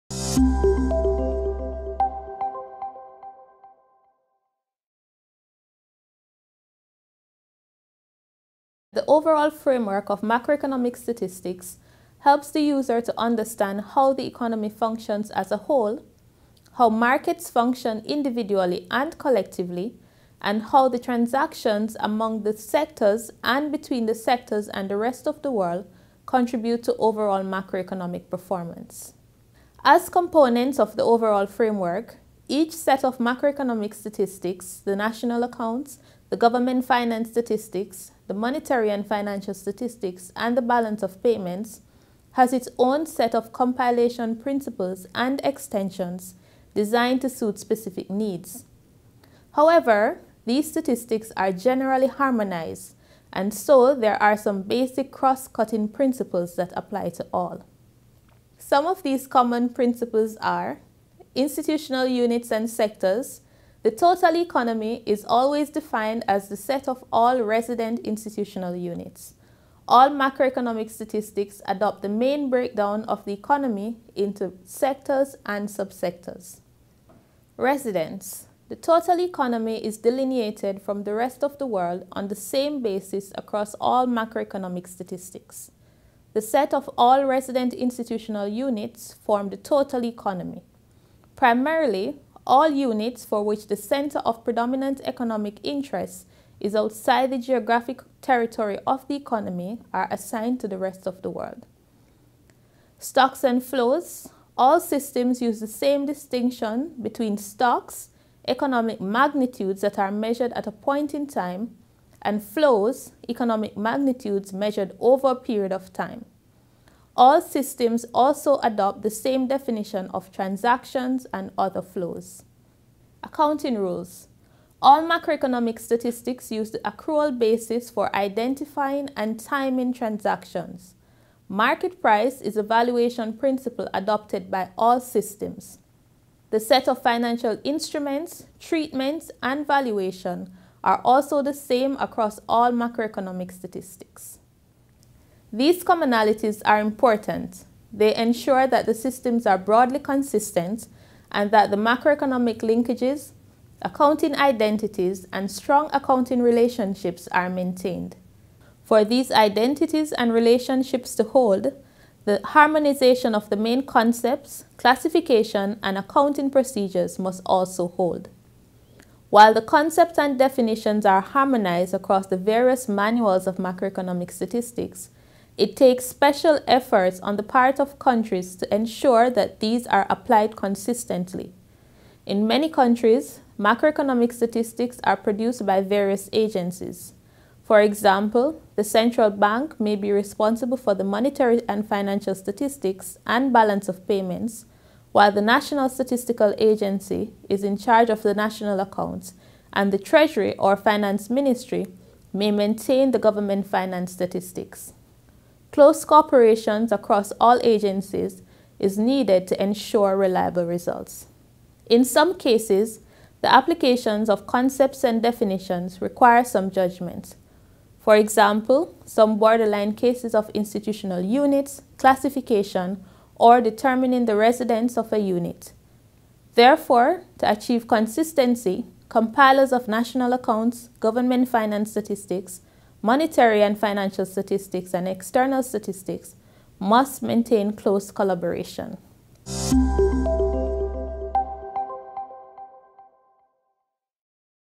An overview of basic compilation principles for macroeconomic statistics
This video provides an overview of the basic compilation principles for the main accounts of macroeconomic statistics—national accounts, balance of payments (BOP) and international investment position (IIP), government finance statistics (GFS), and monetary and financial statistics (MFS). To ensure consistency and allow reconciliation within the integrated framework of these accounts, they are harmonized in terms of definition of residence of institutional units, classification of institutional units into institutional sectors, distinction and integration of stocks and flows of economic values in balance sheets, and accounting rules (accrual accounting of transactions and valuation at market prices).

Link to the IMF online course on compilation basics of macroeconomic statistics for government officials
Link to the IMF online course on compilation basics of macroeconomic statistics for members of the general public